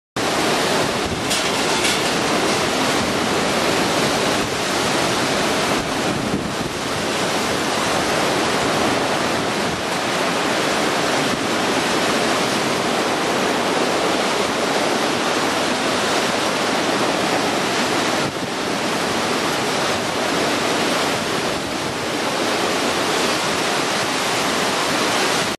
大瀬神社から富士山を見る
２０１２年１２月８日撮影。大瀬崎の神社海岸から。
当日は強風２０ｍ程度でした。